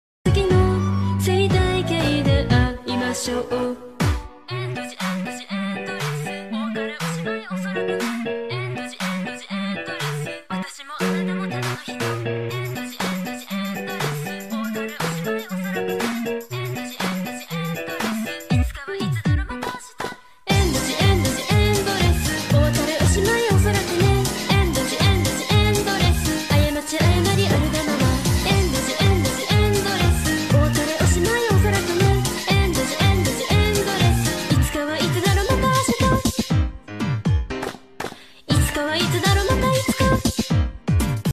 Endless meme
The character in the animation is an oc. His name is Nebojša. I know that there are a few little mistakes so I hope you don't mind. ^^'